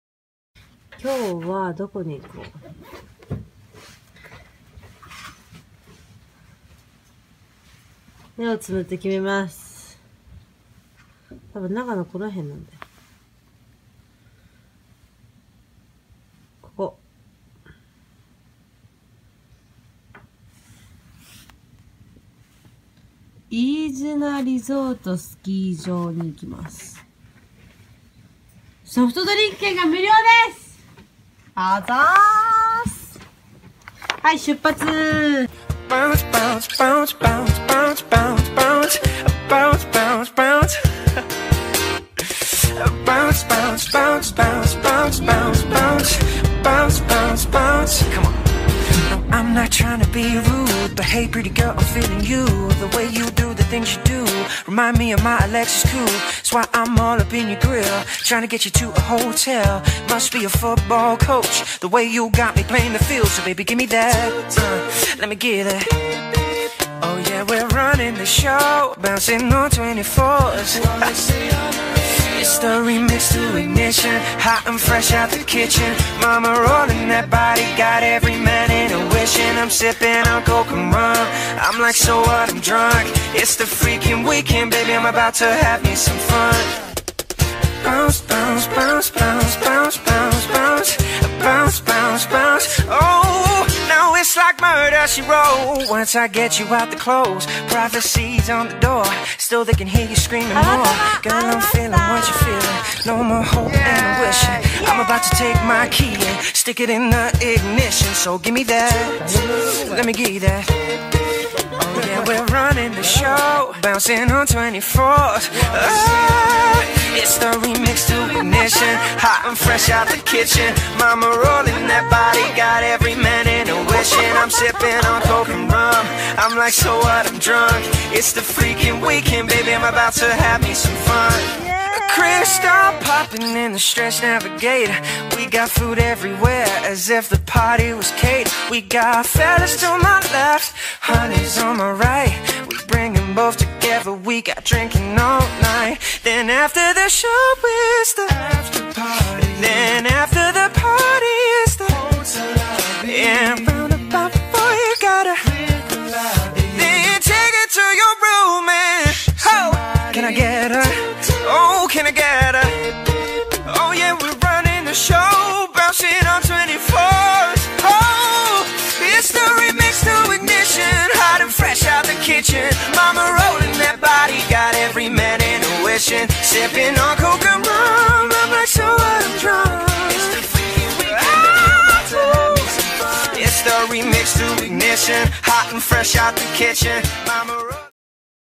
Who's TV #14 【いいづなリゾートスキー場は嬉しい特典つきだった♪】
長野県のいいづなリゾートスキー場へ行ってきました！

雪がまだ少ない時期でしたが
JTB(３０山共通シーズン券)持っているとドリンク券がついてくる嬉しい特典でした♫